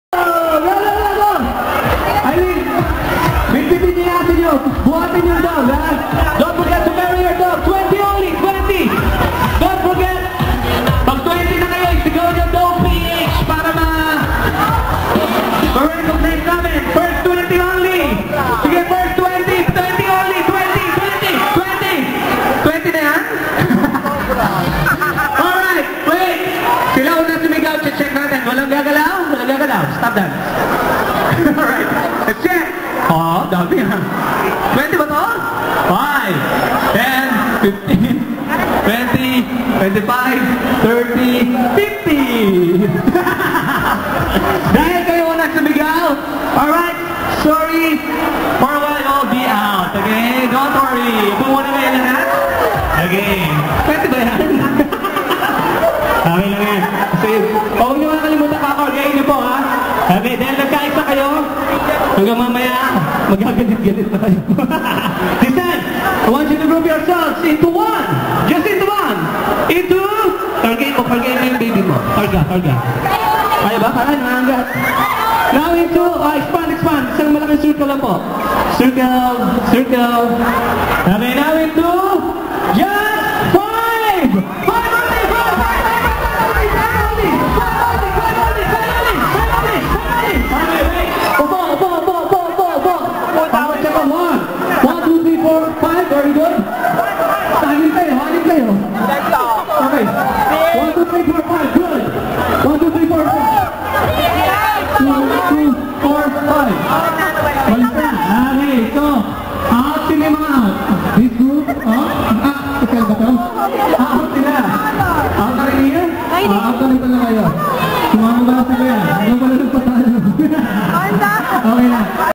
AN EXCERPT FROM DOG PH 1ST YEAR ANNIVERSARY @ SM CITY SAN LAZARO
Hosted by: Renjo Santos
October 26'2013
WE LOVE DOGS
PRAISE GOD!